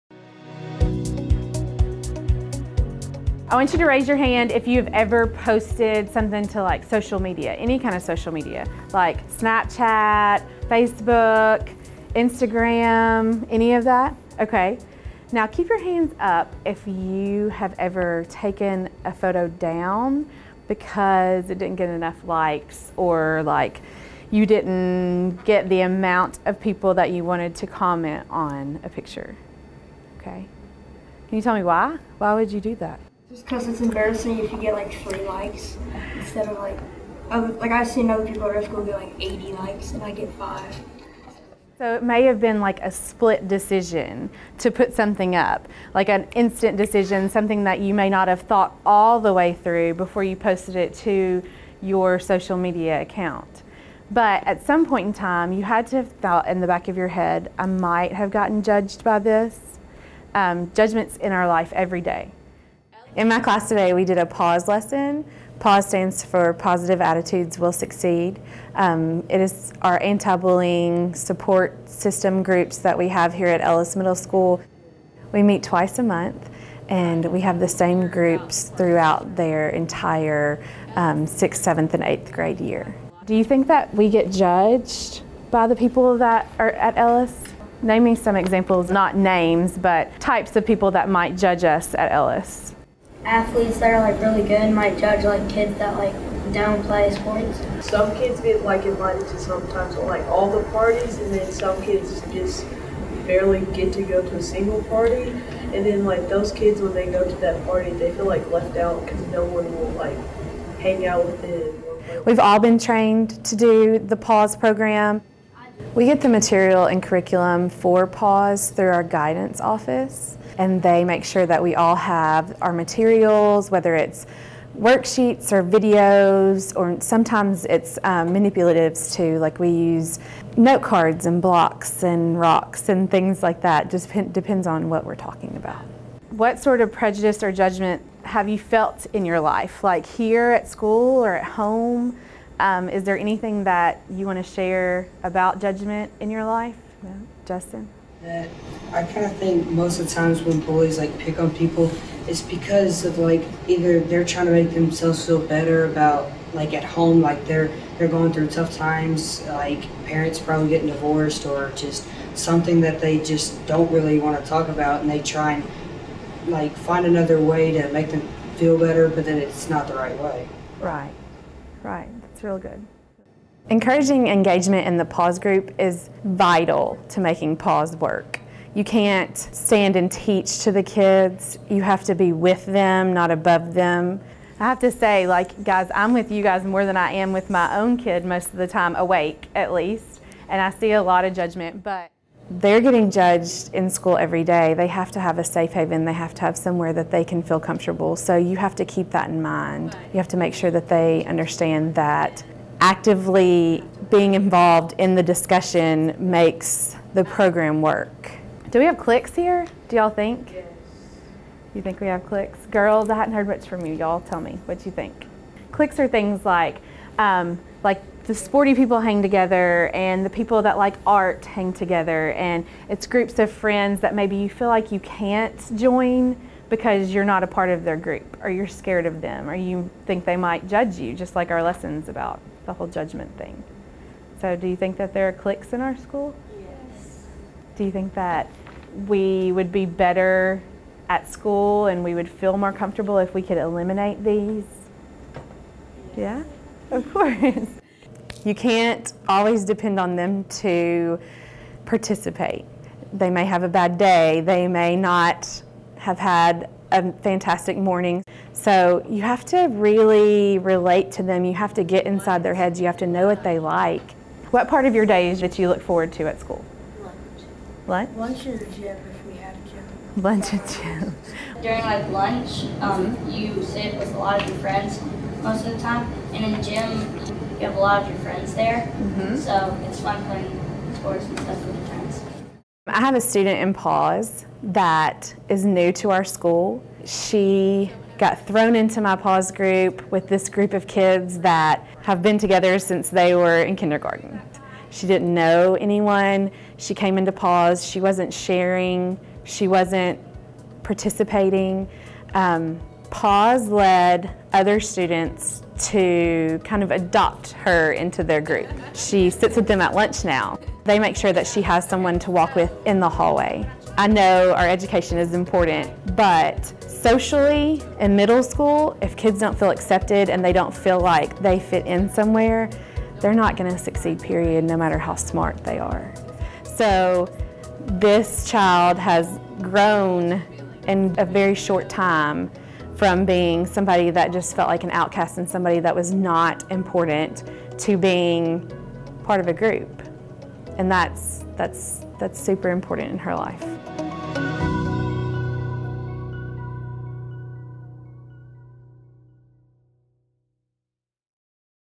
Warmth and Support (Secondary)
Cindy Craig holds an advisory group (called PAWS) with her middle school students. This video was developed for the Tennessee Department of Education.